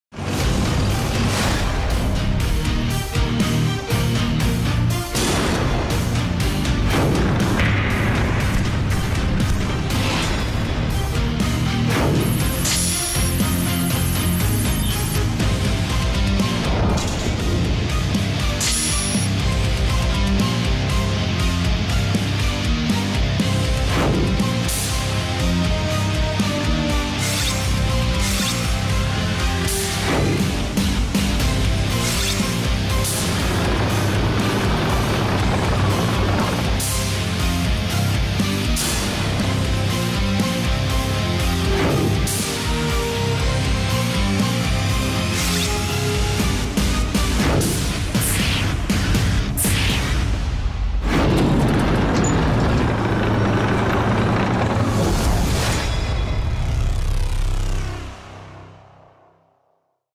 혼합파워팩(국산엔진, 냉각기 + 독일 변속기)을 장착한 K2 전차 2차 양산 출고식 영상 / 현대로템
전투중량 56톤, 120mm 55구경장 주포에 수심 4.1m까지 잠수할 수 있는 3.5세대 전차인 K2의 2차 양산 출고식 영상입니다.  2차 양산에는 1,500마력의 혼합파워팩을 장착해 부품의 해외 의존도를 줄임으로써 K2 전차의 수출경쟁력도 높아졌다고 합니다.